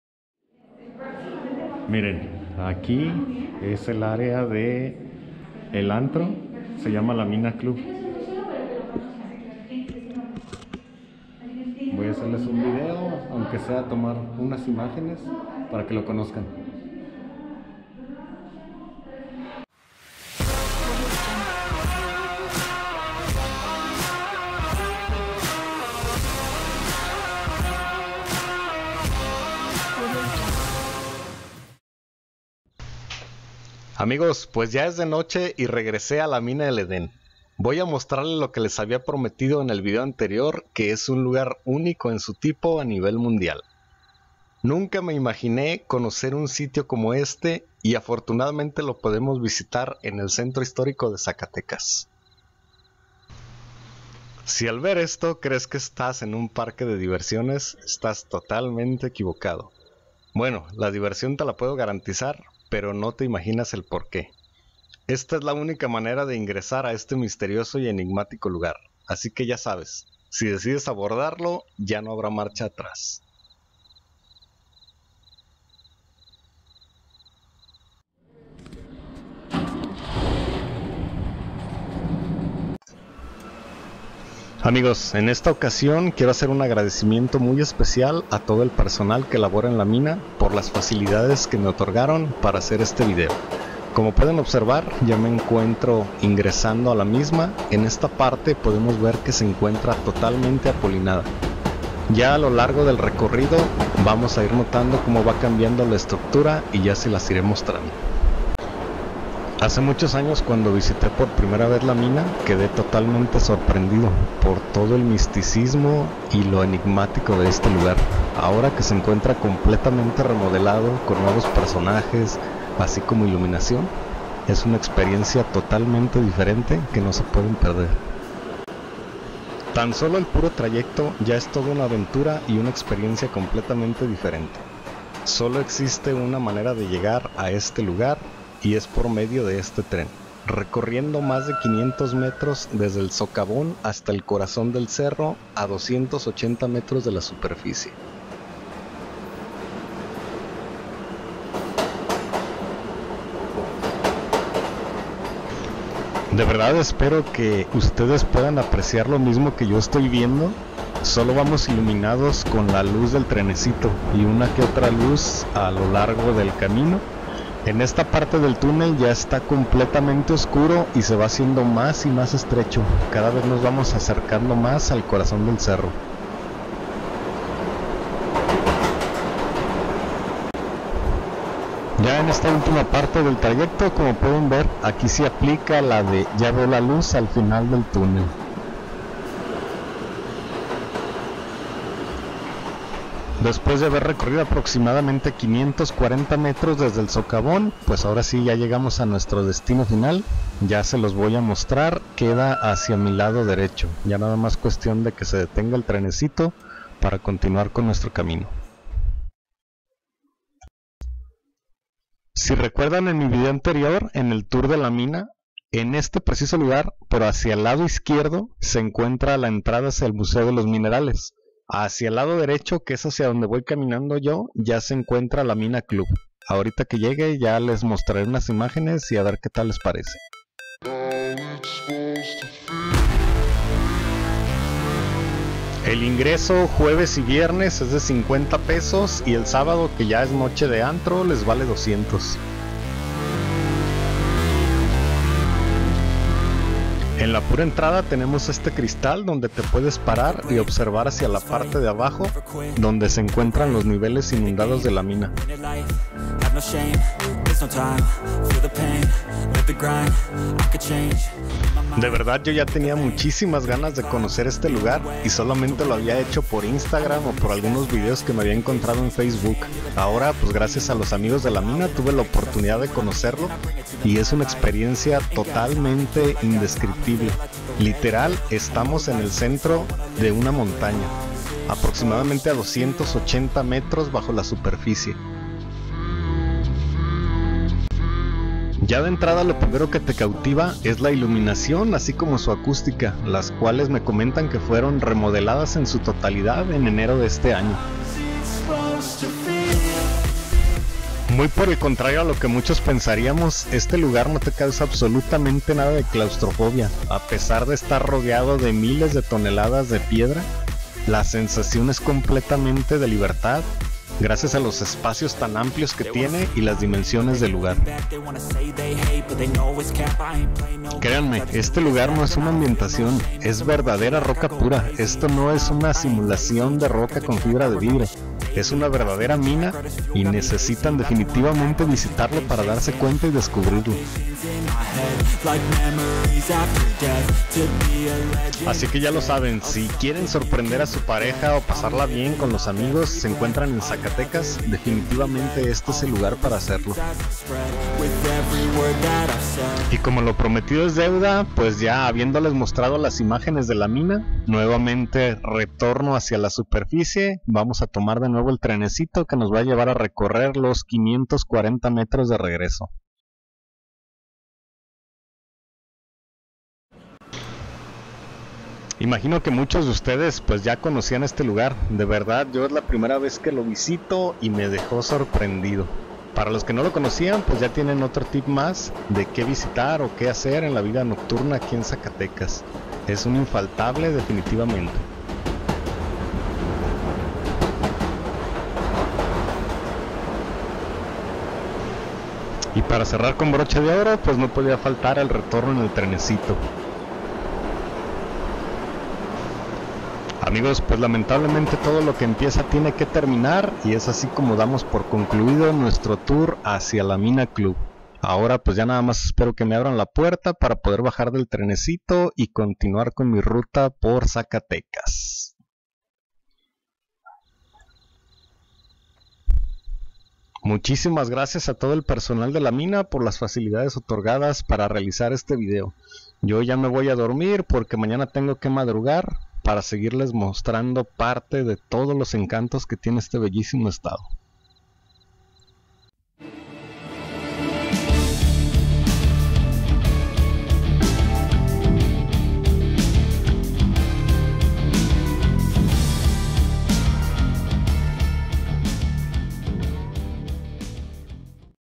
INCREIBLE ANTRO BAR DENTRO DE UNA MINA, UNICO EN EL MUNDO Y LO TENEMOS EN ZACATECAS "LA MINA CLUB"
Hay sitios que si te contaran que existen, simplemente no lo creerías y este es uno de ellos. Alguna vez pensaste que podría existir un antro bar o discoteca en una mina?. Pues si existe y lo tenemos en nuestro México el Bellísimo estado de Zacatecas.
Este increíble lugar se encuentra en el mismísimo corazón de una montaña dentro de los túneles de una muy antigua mina, para llegar solo existe una forma y es por medio de un transporte muy peculiar.
Descubre otro mas de los secretos de Zacatecas y sorpréndete con este enigmático único e increíble lugar.